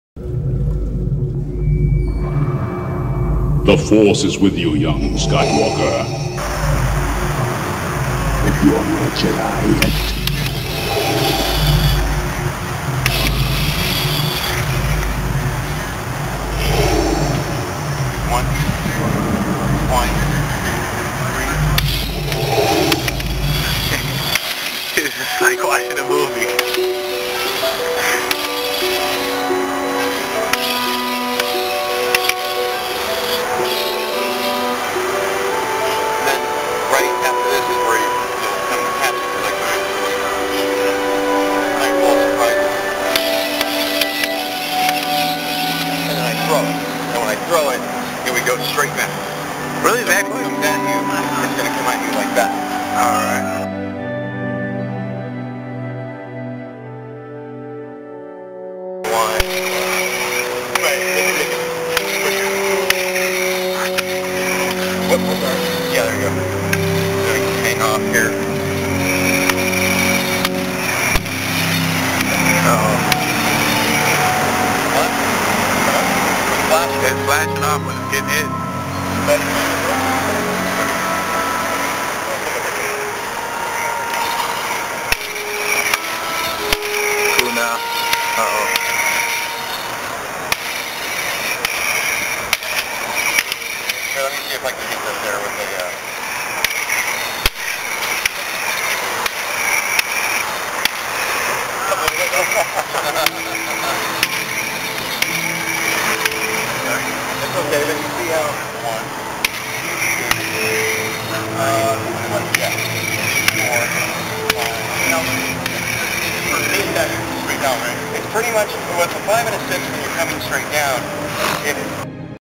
Yoda Vs Bandit VS Corben Duels
First we got Yoda From Fx-sabers Vs Bandit-Jedi (Anakin) practicing the Luke VS Vader ESB duel.
Then Yoda takes out his custom graflex saber and deuls with Corben das and cOrgens custom sith saber (more)